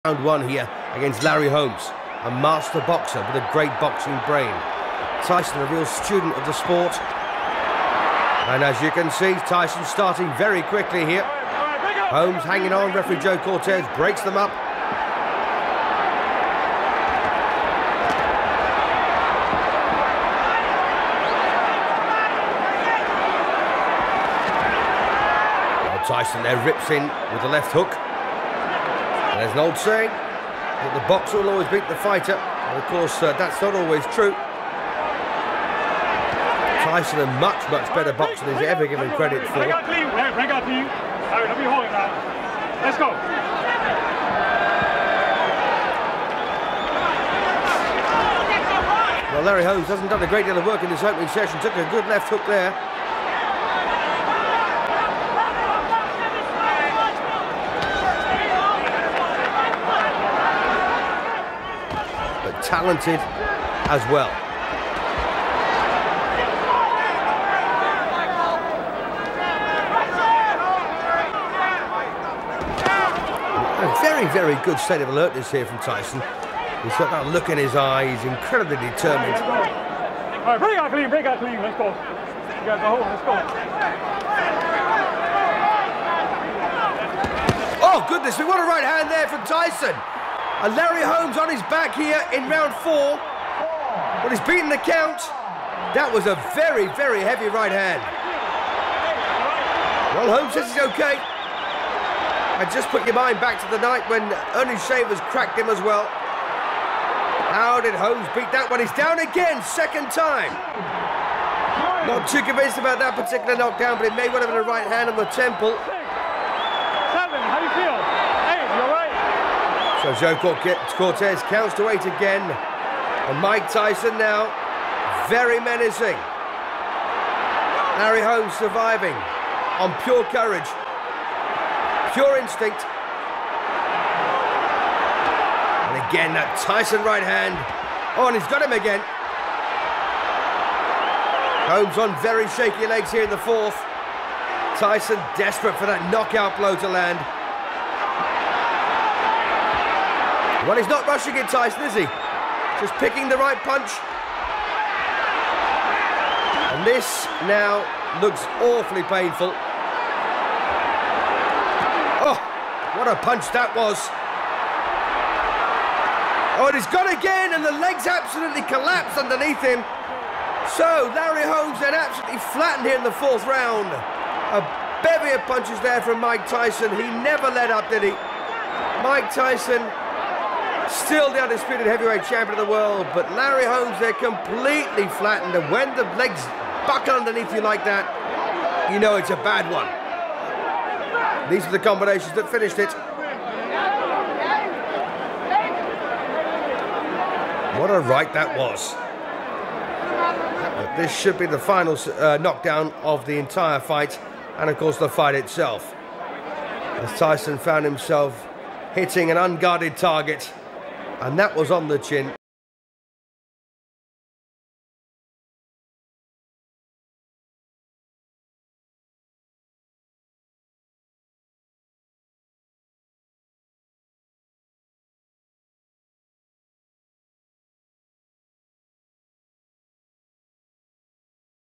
Mike Tyson (USA) vs Larry Holmes (USA) | Boxing Highlights (1988)
Dive into the boxing archives with highlights from the 1988 showdown between USA's Mike Tyson and Larry Holmes, two heavyweight giants! 🥊 Relive the explosive moments as Tyson and Holmes face off in the ring. Don't miss this legendary clash!